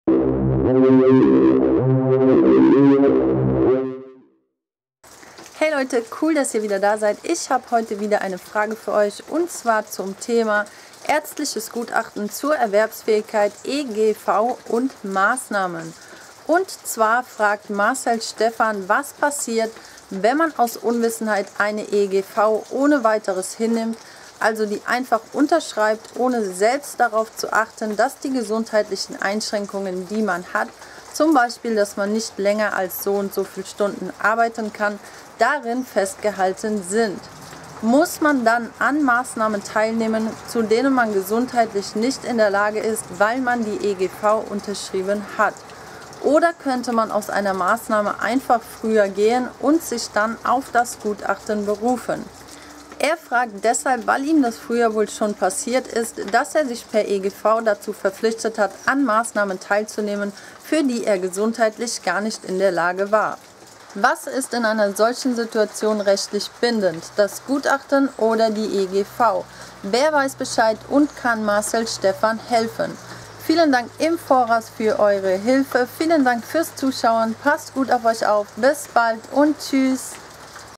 Ärztliches Gutachten oder EGV - was ist bindend für Maßnahmen vom Jocbenter? | Friend in Need!
Was passiert, wenn man ein ärztliches Gutachten hat, gemäß dem die Erwerbsfähigkeit eingeschränkt ist, diese Einschränkungen aber nicht in der Eingliederungsvereinbarung (EGV) festgehalten sind? Ist man als Leistungsbezieher dann verpflichtet, Maßnahmen im Rahmen der EGV teilzunehmen, auch wenn man gesundheitlich dazu nicht in der Lage ist? Oder kann man Maßnahmen mit Verweis auf das ärztliche Gutachten ablehnen oder abbrechen, auch wenn das Jobcenter einen zur Teilnahme zwingen will?